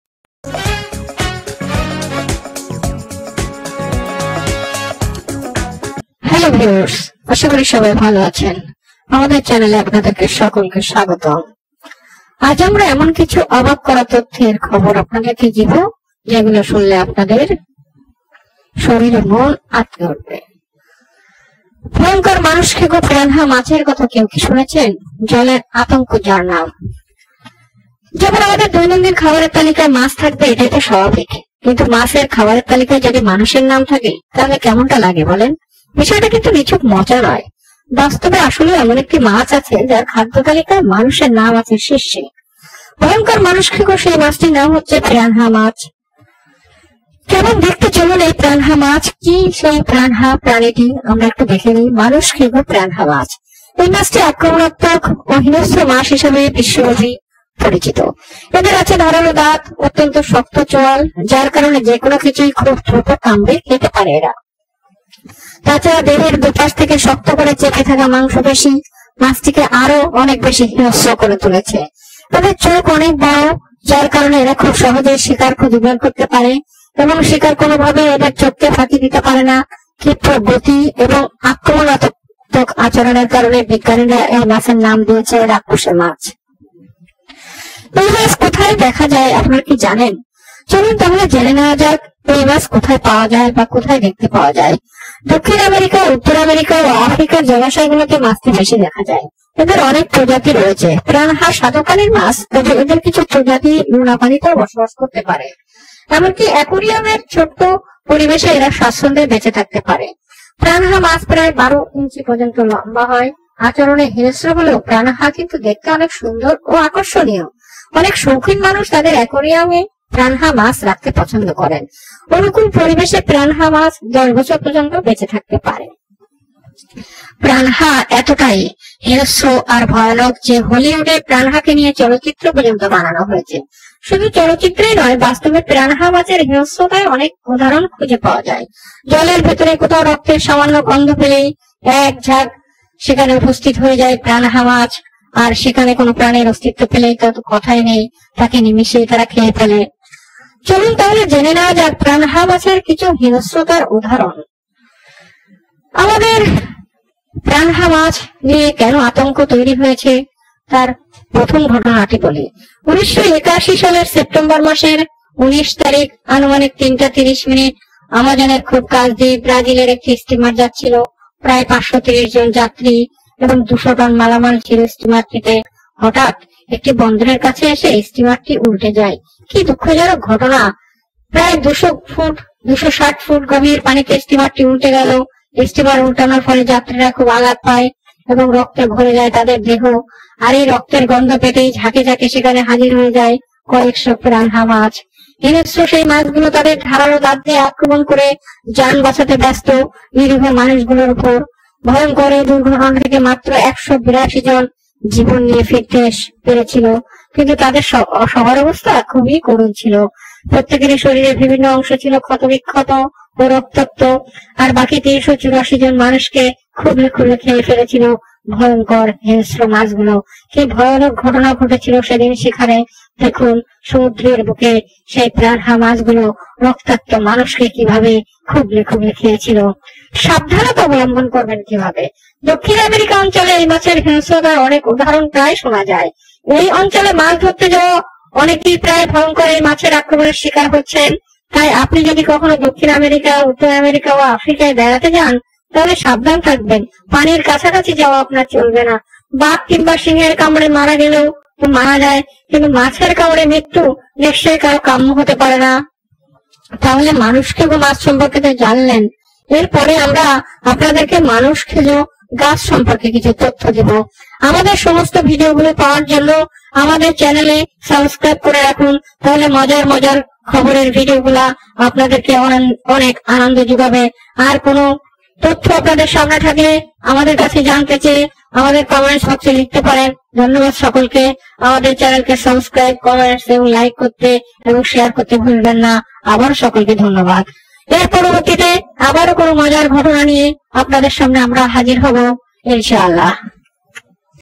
ভয়ংকর মানুষ খেকো পিরানহা যেভাবে খায় মানুষ Piranha Fish Mysterious World কিভাবে পিরানহা মানুষ খা
ভয়ংকর মানুষখেকো পিরানহা মাছ, জলের আতংক যার নাম

আমাদের দৈনন্দিক খাবের তালিকায় মাছ থাকবে এটাই স্বাভাবিক। কিন্তু মাছের খাবারের তালিকায় যদি মানুষের নাম থাকে তাহলে? বিষয়টা কিন্তু নিছক মজা নয়। বাস্তবে আসলেও এমন একটি মাছ আছে যার খাদ্য তালিকার মানুষের নাম আছে শীর্ষে। ভয়ংকর মানুষখেকো সেই মাছটির নাম হচ্ছে পিরানহা মাছ।
মানুষখেকো পিরানহা মাছ
এই মাছটি আক্রমণাত্মক ও হিংস্র মাছ হিসেবে বিশ্বব্যাপী পরিচিত। এদের আছে ধারালো দাঁত ও অত্যন্ত শক্ত চোয়াল যার কারণে যেকোনো কিছুই খুব দ্রুত কামড়ে খেতে পারে এরা। তাছারা দেহের দুপাশ থেকে শক্তকরে চেপে থাকা মাংসপেশী মাছটিকে আরো অনেক বেশি হিংস্র করে তুলেছে। এদের চোখ অনেক বড়। যার কারণে এরা খুব সহজেই শিকার খুঁজে বের করতে পারে এবং শিকার কোনভাবেই এদের চোখকে ফাঁকি দিতে পারে না। ক্ষিপ্রগতি এবং আক্রমণাত্মক আচরনের কারণে বিজ্ঞানীরা এই মাছের নাম দিয়েছে ‘রাক্ষুসে মাছ’।
এই মাছ কোথায় দেখা যায়?
দক্ষিন আমেরিকা, উত্তর আমেরিকা ও আফ্রিকায় জলাশয়গুলোতে মাছটি বেশি দেখা যায়। এদের অনেক প্রজাতি রয়েছে। পিরানহা স্বাদু পানির মাছ তবে এদের কিছু প্রজাতি লোনা পানিতেও বসবাস করতে পারে। এমনকি অ্যাকুয়ারিয়ামের ছোট্ট পরিবেশেও এরা স্বাচ্ছন্দে বেচে থাকতে পারে। পিরানহা মাছ প্রায় ১২ ইঞ্চি পর্যন্ত লম্বা হয়। আচরণে হিংস্র হলেও পিরানহারা কিন্তু দেখতে অনেক সুন্দর ও আকর্ষণীয় হয়। অনেক শৌখিন মানুষ তাদের অ্যাকুরিয়ামে পিরানহা মাছরাখতে পছন্দ করেন। অনুকূল পরিবেশে পিরানহা ১০ বছর পর্যন্ত বেঁচে থাকে।
পিরানহা এতোটাই হিংস্র আর ভয়ানক যে হলিউডে পিরানহাকে নিয়ে চলচ্চিত্র পর্যন্ত বানানো হয়েছে। শুধু চলচ্চিত্রেই নয়, বাস্তবেও পিরানহা মাছের হিংস্রতার অনেক উদাহরণ খুঁজে পাওয়া যায়। জলের ভেতরে কোথাও রক্তের সামান্য গন্ধ পেলেই এরা ঝাঁক বেঁধে সেখানে উপস্থিত হয়। আর সেখানে কোনো প্রাণীর অস্তিত্ব পেলে তো কথাই নেই নিমিষেই তাকে খেয়ে ফেলে।
পিরানহা মাছের হিংস্রতার কিছু উদাহরনঃ
এবার চলুন আপনাদের পিরানহা মাছ নিয়ে সবচেয়ে ভয়ংকর ঘটনাটি বলি। ১৯৮১ সালের সেপ্টেম্বর মাসের ১৯ তারিখ আনুমানিক ৩টা ৩০ মিনিটে আমাজনের খুব কাছ দিয়েই ব্রাজিলের একটি স্টিমার যাচ্ছিলো। প্রায় ৫৩০ জন যাত্রী ও ২০০ টন মালামাল ছিল স্টিমারটিতে। হঠাত্ একটি বন্দরের কাছে এসে স্টিমারটি উল্টে যায় ২৬০ ফুট গভীর পানিতে। স্টিমার উল্টানোর ফলে যাত্রীরা খুব আঘাত পায় এবং রক্তে ভরে যায় তাদের দেহ।। আর এই রক্তের গন্ধ পেতেই ঝাঁকে ঝাঁকে সেখানে হাজির হয়ে যায় কয়েকশো পিরানহা মাছ। হিংস্র সেই মাছগুলো তাদের ধারালো দাঁত নিয়ে আক্রমণ করে জান বাঁচাতে ব্যাস্ত নিরীহ মানুষ গুলোর উপর। ভয়ংকর এই দুর্ঘটনা থেকে মাত্র ১৮২ জন জীবন নিয়ে ফিরতে পেরেছিল। কিন্তু তাদের সবার অবস্থাই ছিলো খুব করুন। প্রত্যেকেরই শরীরের বিভিন্ন অংশ ছিল খত বিক্ষত এবং রক্তাক্ত। আর বাকি ৩৮৪ জন মানুষকে খুবলে খুবলে খেয়ে ফেলেছিল ভয়ংকর হিংস্র মাছগুলো!
সাবধান!
দক্ষিণ আমেরিকা অঞ্চলে এই মাছের হিংস্রতার অনেক ঘটনার কথা প্রায়ই শোনা যায়। অই অঞ্চলে মাছ ধরতে যাওয়া অনেকেই প্রায়ই ভয়ংকর এই মাছের আক্রমনের শিকার হচ্ছে। তাই আপনি যদি কখনও দক্ষিন আমেরিকা, উত্তর আমেরিকা ও আফ্রিকায় বেড়াতে জান, তাহলে সাবধান থাকবেন! পানির কাছাকাছি যাওয়া চলবে না। বাঘ কিংবা সিংহের কামড়ে মারা গেলও তা মানা যায়। কিন্তু মাছের কামড়ে মৃত্যু! নিশ্চয়ই কারো কাম্য হতে পারে না!
মানুষখেকো মাছ সম্পর্কে তো জানলেন, এবার চলুন মানুষখেকো গাছ সম্পর্কে জেনে নেই।
ভয়ংকর মানুষ খেকো পিরানহা যেভাবে খায় মানুষ |Piranha Fish | Mysterious World |  কিভাবে পিরানহা মানুষ খায় দেখুন